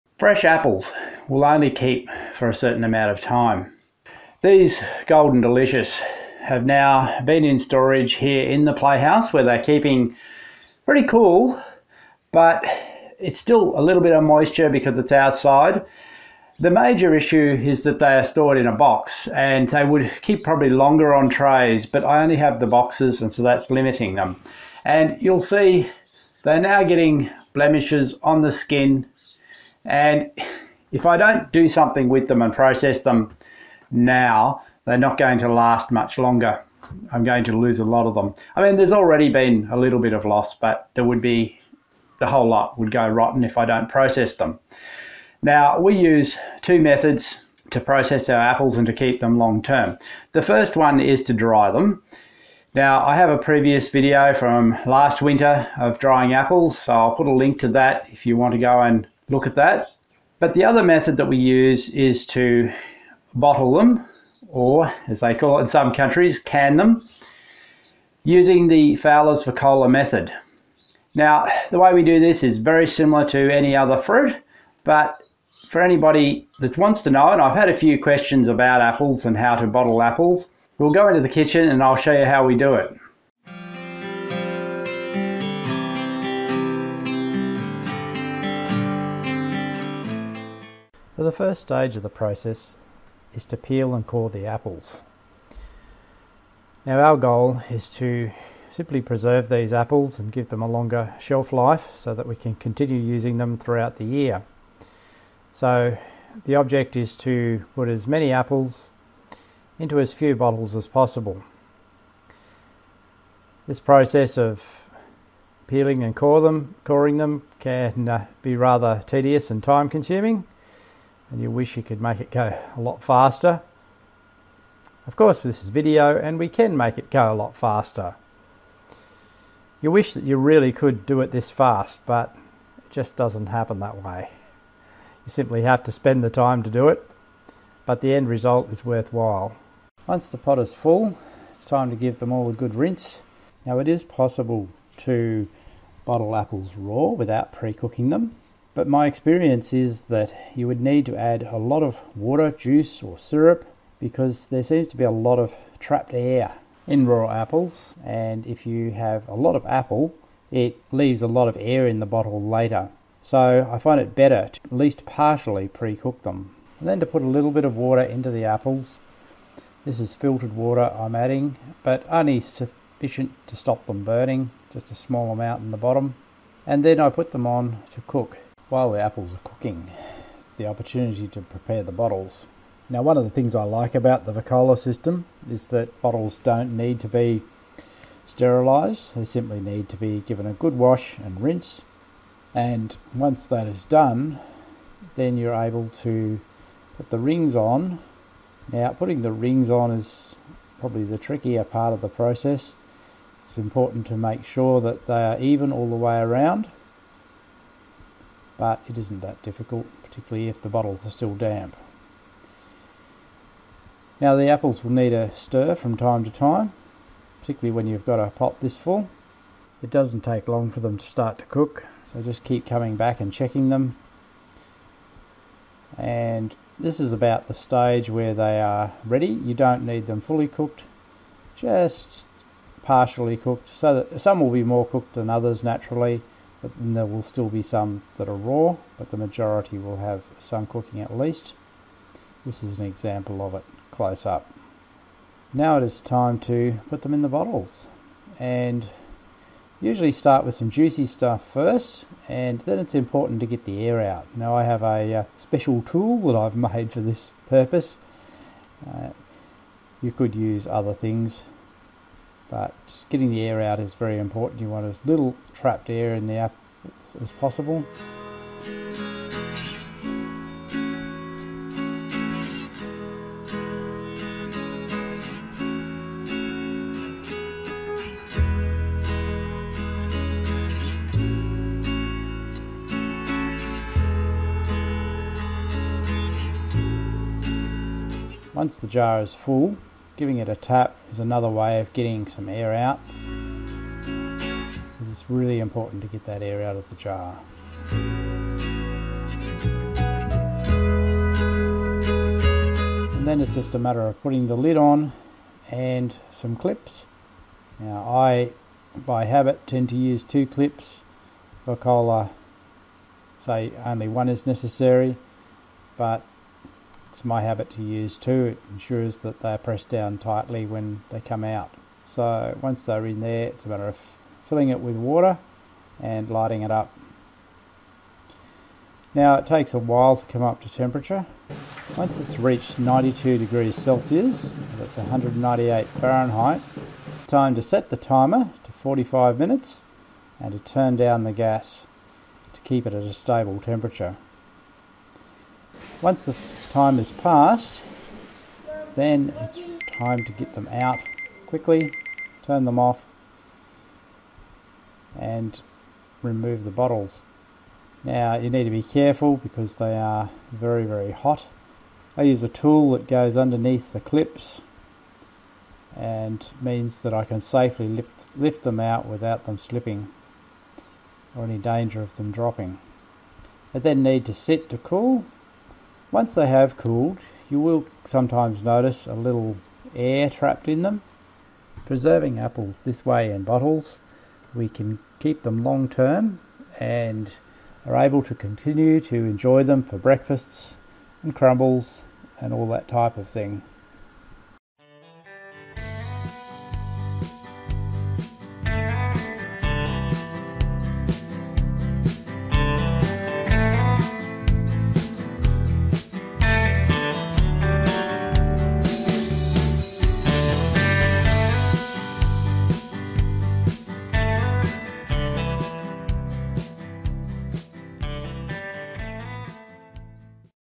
How We Preserve Apples Long Term
Apples will only keep fresh for a limited amount of time - in order to preserve the harvest and keep them long term - which is the key to being self-sufficient -  we bottle them using the Fowlers Vacola method (aka "Canning".)